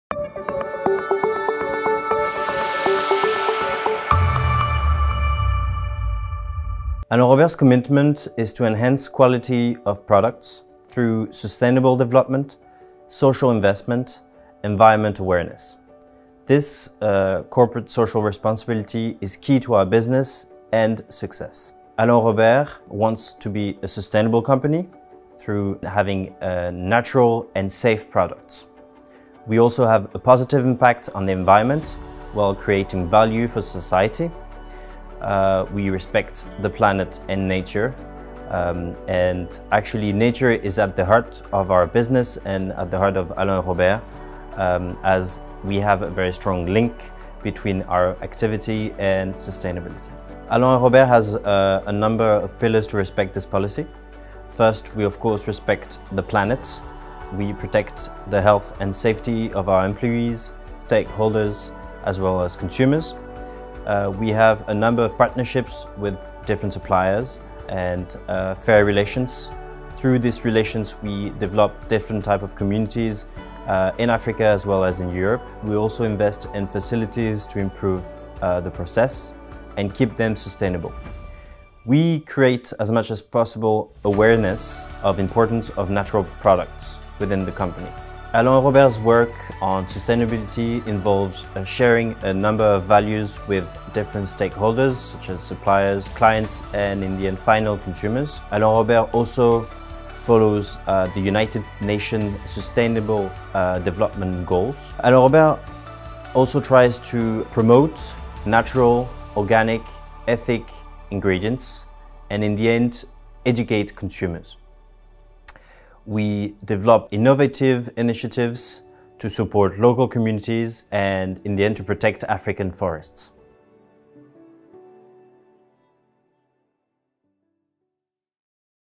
Meet Alland & Robert: sustainability policy
Charles Alland discusses Alland & Robert sustainability policy and underlines its importance for our company.